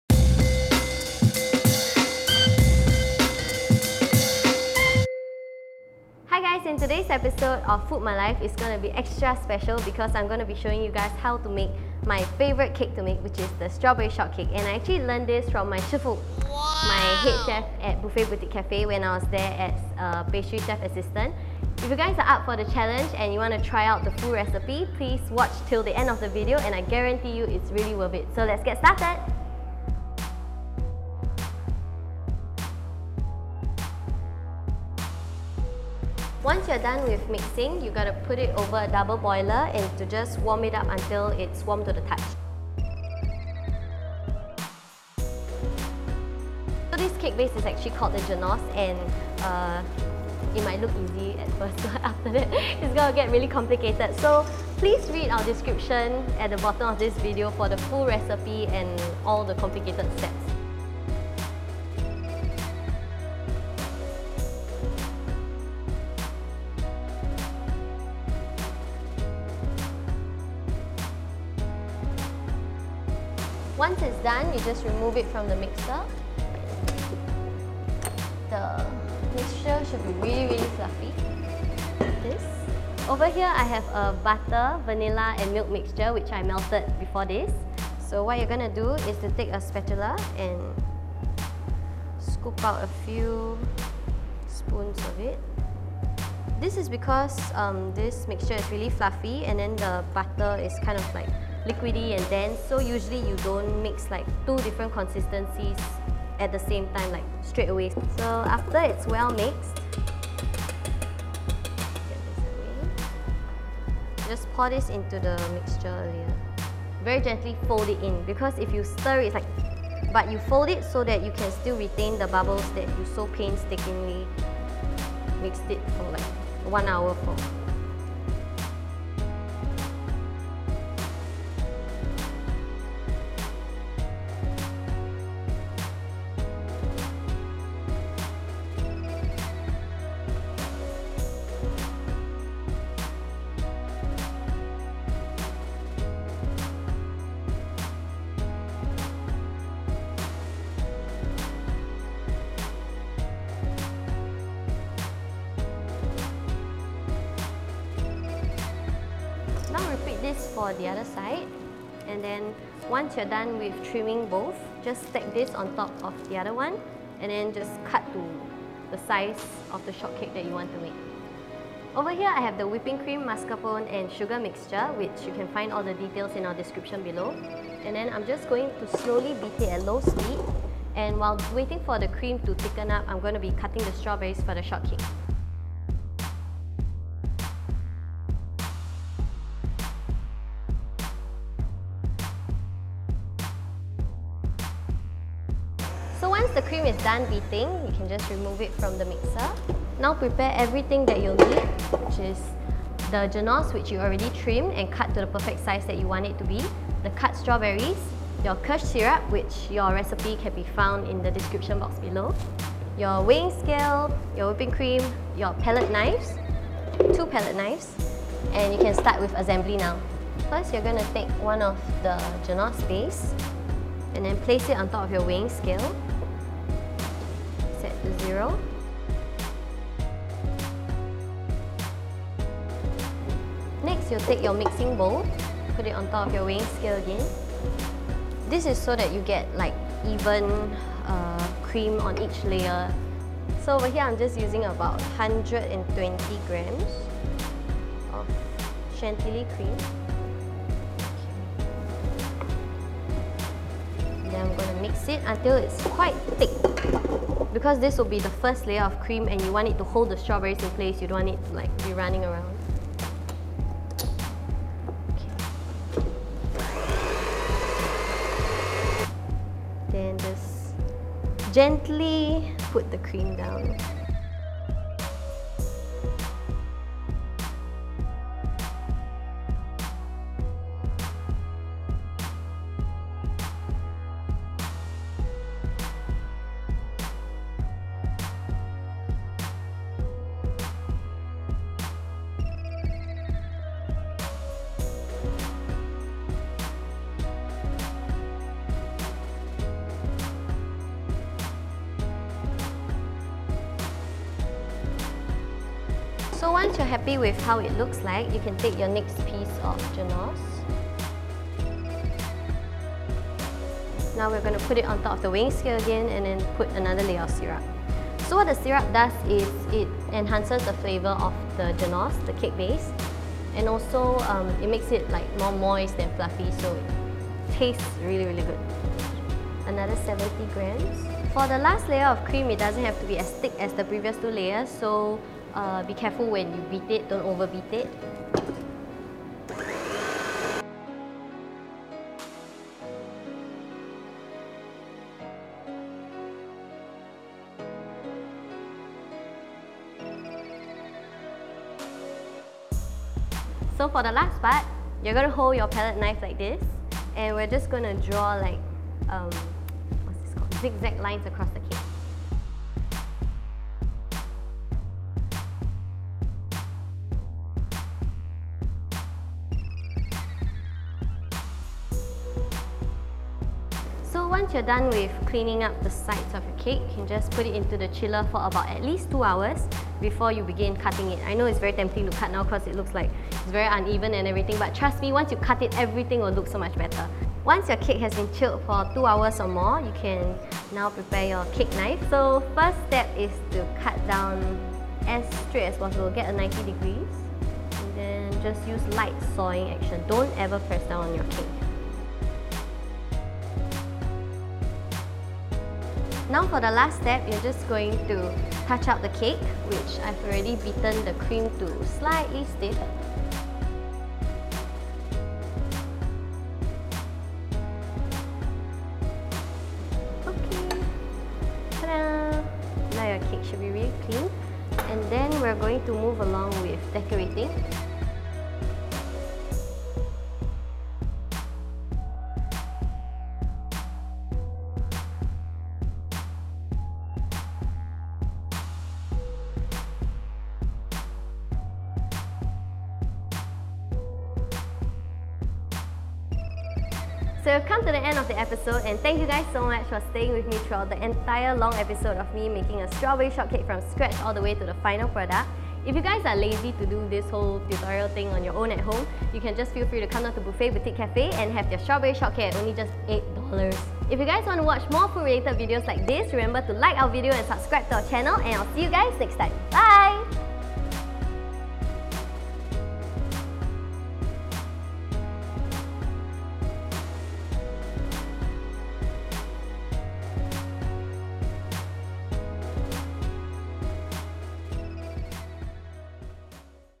EP8 FOOD MY LIFE - Strawberry Shortcake
Rachell shares with how to make professional standard strawberry shortcake. Full recipe below:

Genoise - cake base for Strawberry Shortcake

Ingredients

Eggs 6

Sugar 185g

Cake Flour 178.5g

Milk 40g

Butter 22.5g

Vanilla 3.5g

Steps

    Whisk together (Eggs + Sugar) in a bowl.
    Using a double boiler, heat up mixture, constantly whisking to prevent eggs from cooking at the bottom.
    Continue whisking and heating on double boiler until a bit mixture feels warm to the touch, approx 50deg C.
    Pour mixture into mixing bowl with whisk attachment. Beat on high speed for 4 minutes, beat on medium speed for 15 minutes, then beat on low speed for 30 minutes.
    In a separate pot, melt together (milk + butter + vanilla).
    Take one cup of batter from Step 4 and mix into melted - milk + butter + vanilla - mixture.
    Fold mixture from Step 6. into remaining batter from Step 4.
    Sift flour gently and fold until even.
    Pour batter into pans lined with baking sheets.
    Smooth out batter to ensure even baking.
    Bake at 230 degC for 8 to 11 minutes.

Kirsh Syrup (for Strawberry Shortcake)

Ingredients

Kirsh Syrup Gel 25g

Sugar Syrup 25g

Water 120g

Steps

    Mix all ingredients well

Chantily Cream (for Strawberry Shortcake)

Ingredients

Mascarpone 50g

Sugar 40g

Steps

    Whisk on medium speed until slightly thick and creamy.

Weigh out and whisk the Chantilly Cream separately to get desired consistency for each layer.

 Bottom most layer: Stiff

 Layer above strawberries: Less Stiff

 Top layer: Smooth

Steps to Assemble Strawberry Shortcake:

1. Genoise + Kirsch Syrup
2. Chantilly Cream (Stiff)
3. Strawberries
4. Chantilly Cream (Less Stiff)
5. Genoise + Kirsch Syrup
6. Chantilly Cream (Smooth)
7. Decorate as you'd like!